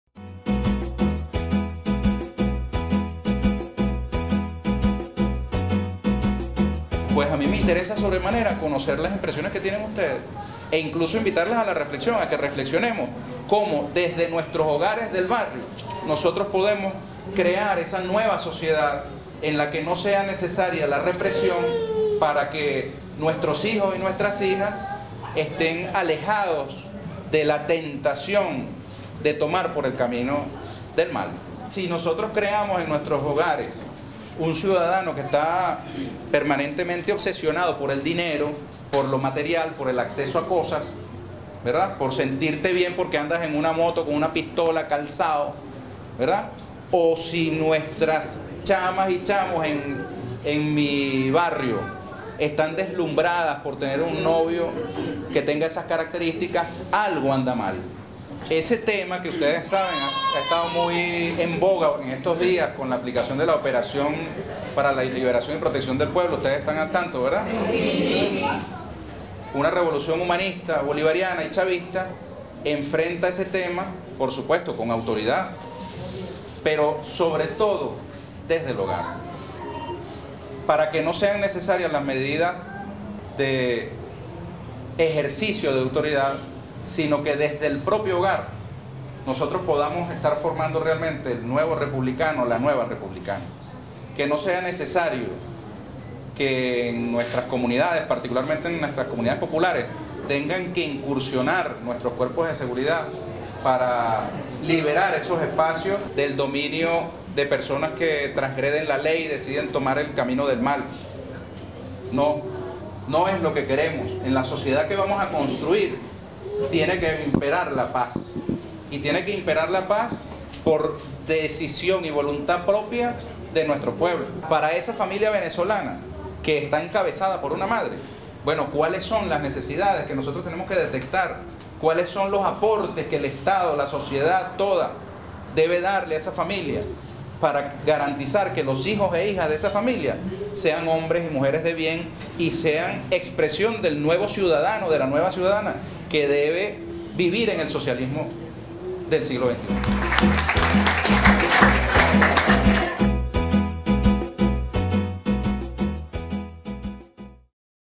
Villegas invita a prevenir la criminalidad con valores en el hogar
Este miércoles, el candidato a la Asamblea Nacional por el circuito 1 de Caracas, Ernesto Villegas, asistió como invitado al Taller de Formación de las Madres del Barrio y Hogares de la Patria, realizado en la sede de la Universidad Nacional Experimental de la Seguridad (UNES) en la parroquia Sucre de la ciudad Capital.